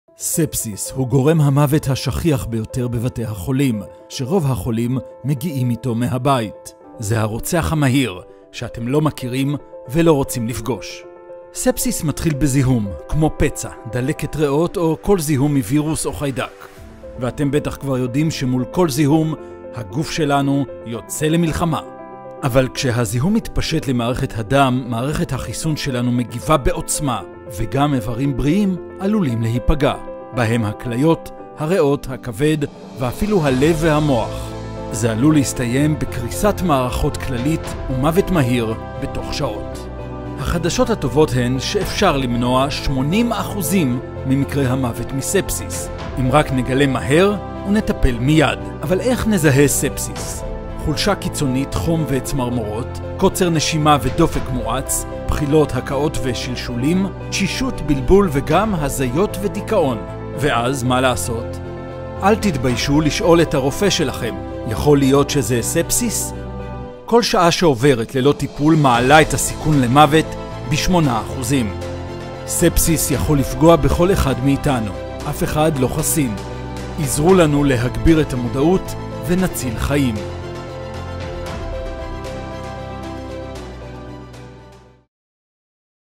זהירות ספסיס! - מה הסימפטומים ומה לעשות
ספסיס הוא גורם המוות השכיח ביותר בבתי החולים, ורוב החולים מגיעים איתו מהבית.
זה הרוצח המהיר – שאתם לא מכירים, ולא רוצים לפגוש.
ספסיס מתחיל בזיהום. כמו פצע, דלקת ריאות, או כל זיהום מווירוס או חיידק.
ואתם בטח כבר יודעים שמול כל זיהום - הגוף שלנו יוצא למלחמה!
אבל כשהזיהום מתפשט למערכת הדם, מערכת החיסון שלנו מגיבה בעוצמה וגם איברים בריאים עלולים להיפגע.
בהם הכליות, הריאות, הכבד, ואפילו הלב והמוח.
זה יכול להסתיים בקריסת מערכות כללית, ומוות מהיר בתוך שעות.
החדשות הטובות הן שאפשר למנוע שמונים אחוזים ממקרי המוות מספסיס!
אם רק נגלה מהר, ונטפל מיד.
אבל איך נזהה ספסיס?
חולשה קיצונית, חום וצמרמורת,
קוצר נשימה ודופק מואץ,
בחילות, הקאות, ושלשולים,
תשישות, בלבול וגם הזיות ודיכאון.
ואז, מה לעשות?
לפנות מהר לקבלת טיפול רפואי.
אל תתביישו לשאול את הרופא שלכם: יכול להיות שזה ספסיס?
כל שעה שעוברת ללא טיפול מעלה את הסיכון למוות בשמונה אחוזים.
ספסיס יכול לפגוע בכל אחד מאתנו, אף אחד לא חסין.
עזרו להסתדרות הרפואית בישראל להגביר את המודעות – ונציל חיים.
מוגש על ידי ההסתדרות הרפואית בישראל למען בריאות הציבור.